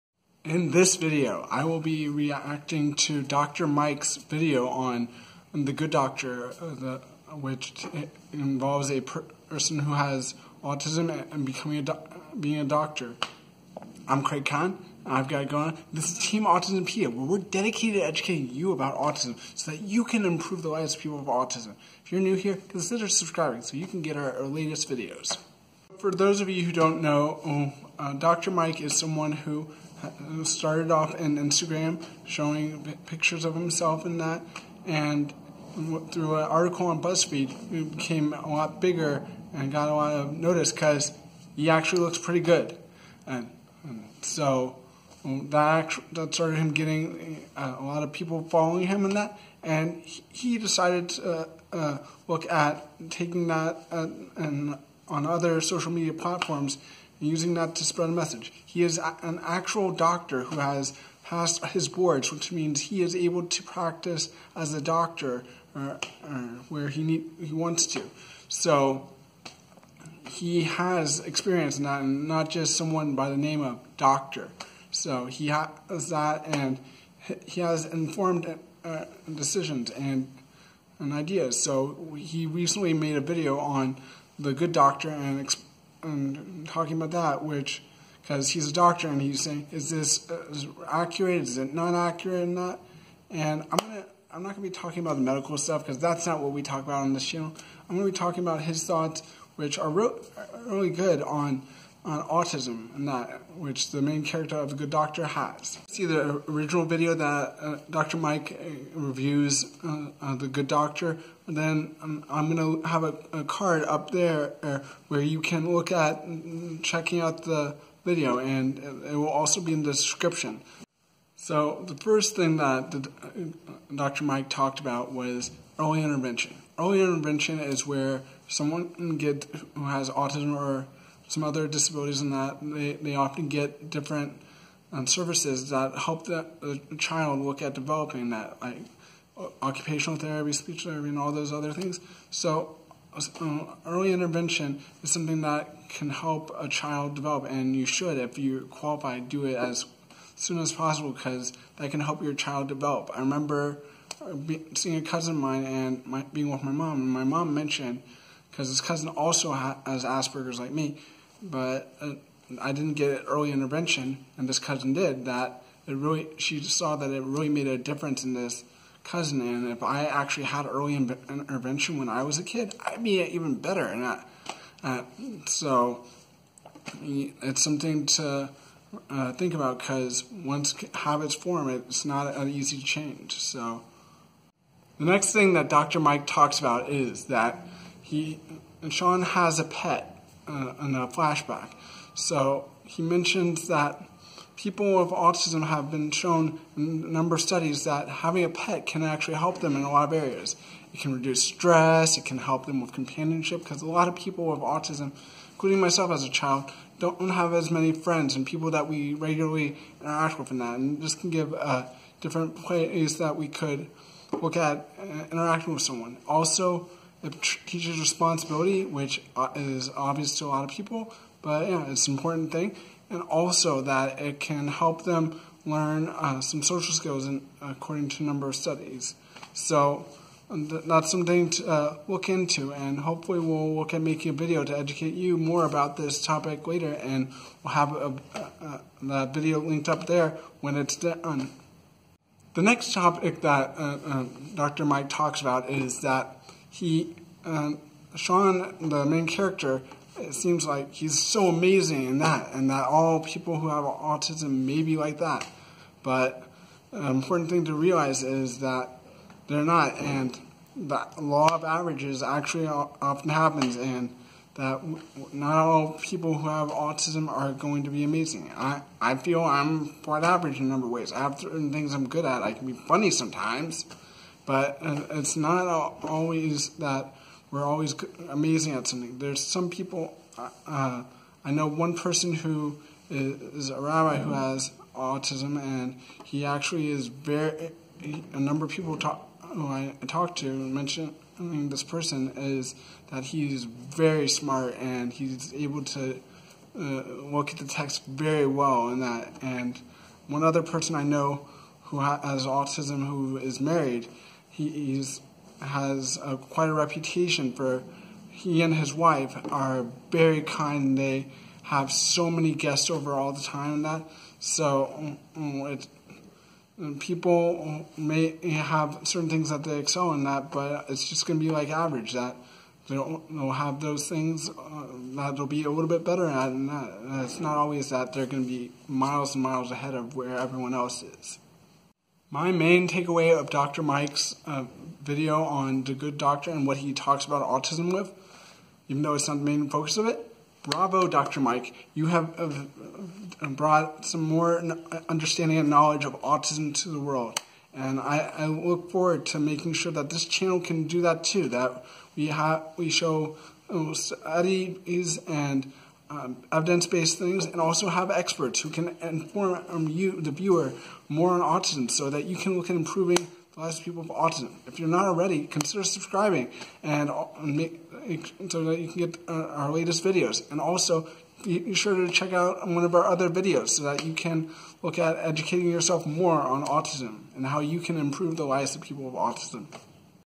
My reaction Dr Mike reacts to the Good Doctor (Just Autism)
Doctor Mike just reacted to the Good doctor and shared some things about autism.  Here is what I think about it.

Doctor Mike is a board certified doctor who blew up on Instagram and is now on all social media.  He recently started a series where he watches a medical drama like Grey's Anatomy, and in the video we are focusing on he talks about The Good Doctor.  The Good Doctor deals with Shaun Murphy who is doing his residency at the prestigious San Jose St. Bonaventure Hospital.  The show focuses on how Shaun deals with his autism in his life having to prove himself, and deal with challenges because of his autism.

Doctor Mike talks about what he sees in the show as a doctor like is the show accurately portraying things medically.  He also talks about autism which I elaborate on and give a little more explanation on.